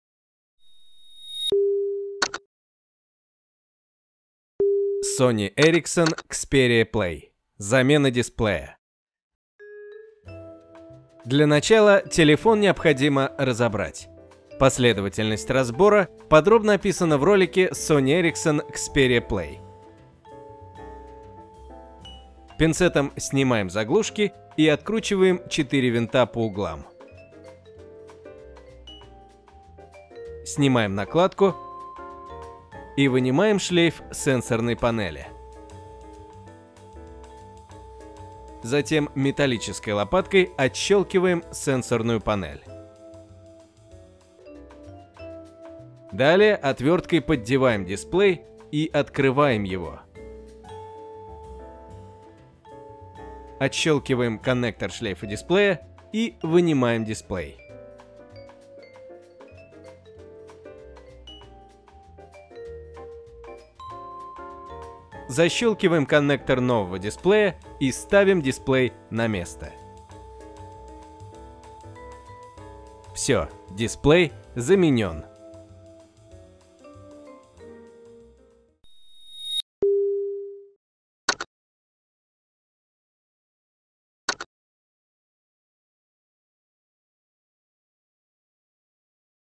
Ремонт Sony Ericsson Xperia PLAY - замена дисплея в телефоне
Инструкция по замене экрана дисплея LCD в сотовом телефоне Сони Эриксон Иксперия Плей: как заменить экран своими руками, если он не работает.